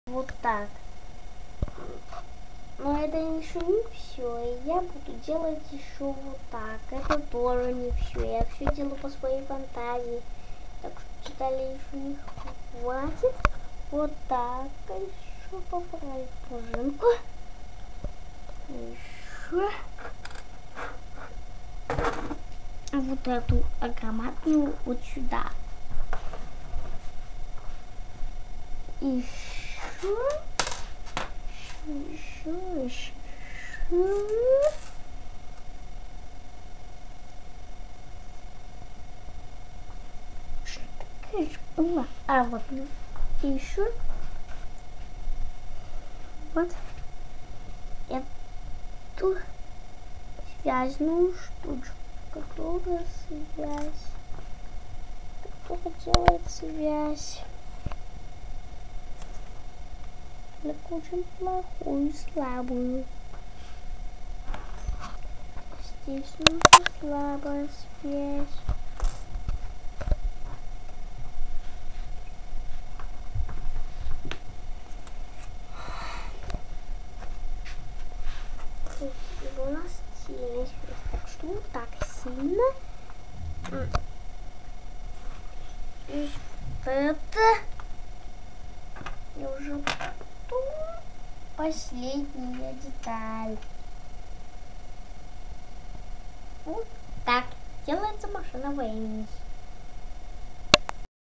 Как сделать машину времени собственными руками.
Самостоятельное создание машины времени из подручных материалов собственными руками дома.
Данила Дмитриевич рассказывает и показывает как легко сделать машину времени из доступных каждому деталей.